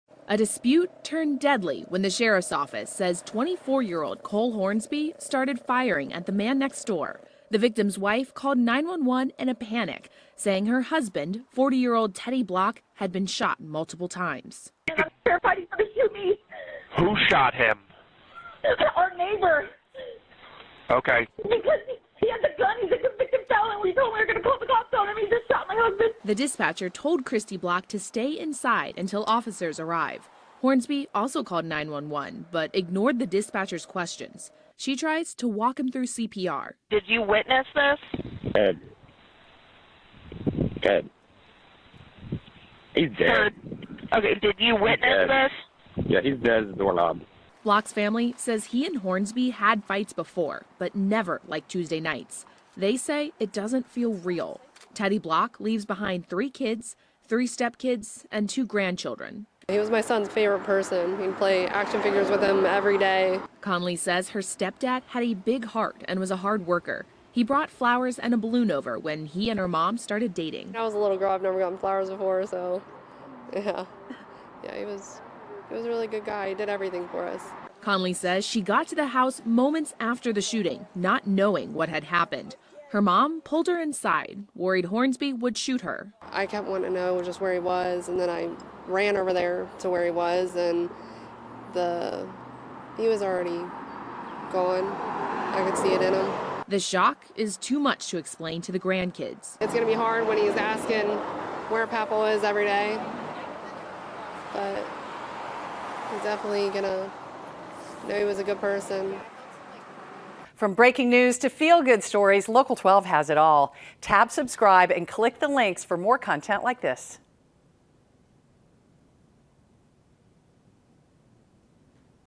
Greater Cincinnati man accused of gunning down neighbor following argument
A Cincinnati man is accused of shooting and killing his neighbor